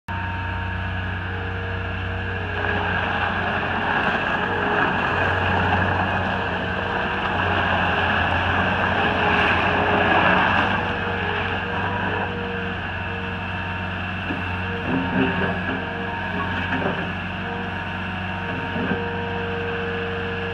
2014 Volvo MC85 Skid Steer #2191
2014 Volvo MC85C Skid Steer #2191 Enclosed Cab, Heat/Air Conditioning, Hand Controls, Quick Connect, Auxiliary Hydraulics, Diesel, 1750 Lift, 56HP, 60” Tooth Bucket, Side Entry, 790 Hours